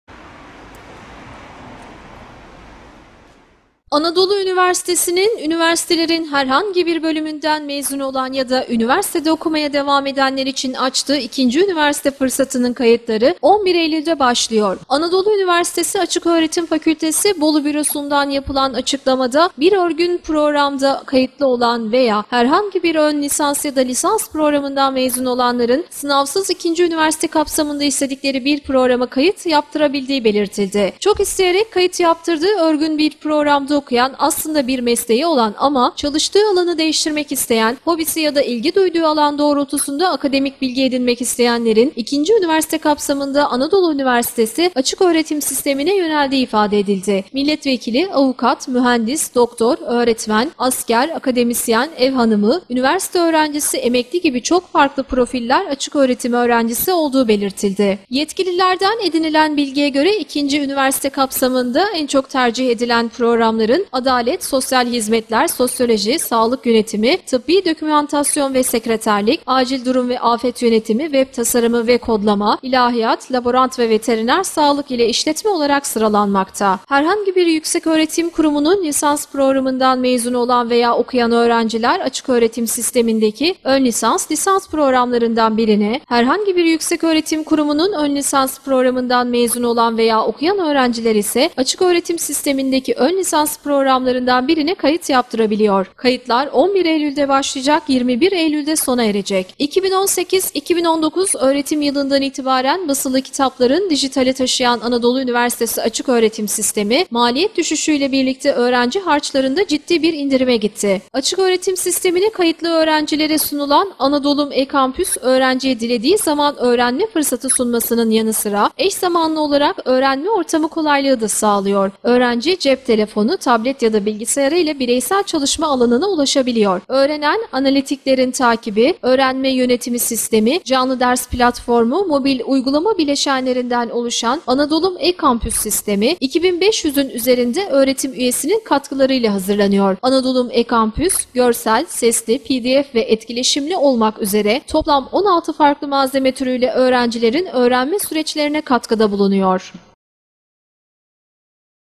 KAYITLAR 11 EYLÜL’DE BAŞLIYOR (15.09.2018)
Anadolu Üniversitesi'nin (AÜ), üniversitelerin herhangi bir bölümünden mezun olan ya da üniversitede okumaya devam edenler için açtığı ikinci üniversite fırsatının kayıtları 11 Eylül'de başlıyor.